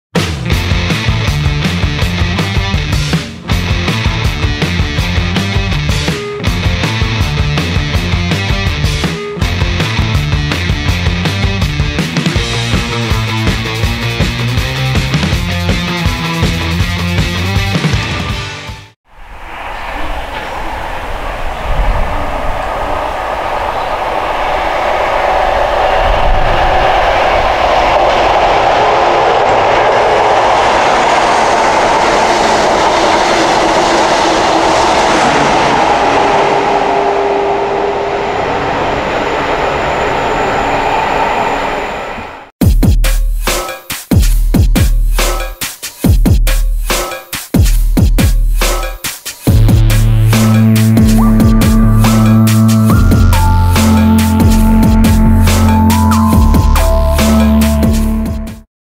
43155 + 43154 Teignmouth 101022
43155 "Rougemont Castle", with 43154 "Compton Castle" on the rear, passes through Teignmouth with 2U16 08.50 Penzance - Cardiff Central on 10th October 2022.